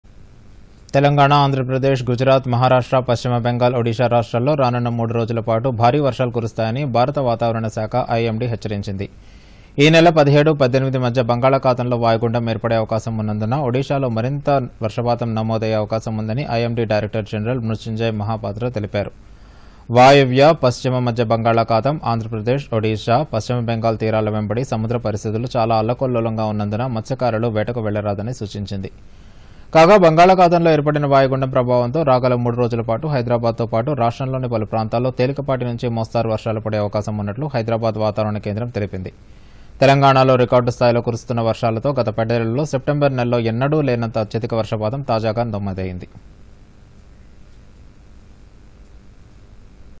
Indian Meteorological Department (IMD) alerts | 🟥 DD News Telangana
తెలంగాణ, ఆంధ్రప్రదేశ్,గుజరాత్, మహారాష్ట్ర, పశ్చిమ బెంగాల్, ఒడిశా రాష్ట్రాల్లో రానున్న మూడు రోజుల పాటు భారీ వర్షాలు కురుస్తాయని భారత వాతావరణ శాఖ (ఐఎండీ) హెచ్చరించింది. ఈ నెల  17-18 మధ్య బంగాళాఖాతంలో వాయుగుండం ఏర్పడే అవకాశం ఉన్నందున ఒడిశాలో మరింత వర్షపాతం నమోదయ్యే అవకాశం ఉందని IMD డైరెక్టర్ జనరల్ మృత్యుంజయ్ మహపాత్ర తెలిపారు.  వాయువ్య,  పశ్చిమ మధ్య బంగాళాఖాతం,  ఆంధ్రప్రదేశ్, ఒడిశా,  పశ్చిమ బెంగాల్ తీరాల వెంబడి సముద్ర పరిస్థితులు చాలా అల్లకల్లోలంగా ఉన్నందున మత్స్యకారులు వేట‌కు వెళ్ల‌రాద‌ని సూచించింది.  కాగా.. బంగాళాఖాతంలో ఏర్పడిన వాయుగుండం ప్రభావంతో రాగల మూడు రోజుల పాటు హైదరాబాద్ తో పాటు రాష్ట్రం లోని పలు ప్రాంతాల్లో తేలికపాటి నుంచి మోస్తరు వర్షాలు పడే అవకాశం ఉన్నట్టు హైదరాబాద్‌ వాతావరణ కేంద్రం తెలిపింది. తెలంగాణలో రికార్డుస్థాయిలో కురుస్తున్న వర్షాలతో.. గత పదేళ్లలో సెప్టెంబరు నెలలో ఎన్నడూలేనంత అత్యధిక వర్షపాతం తాజాగా నమోదైంది.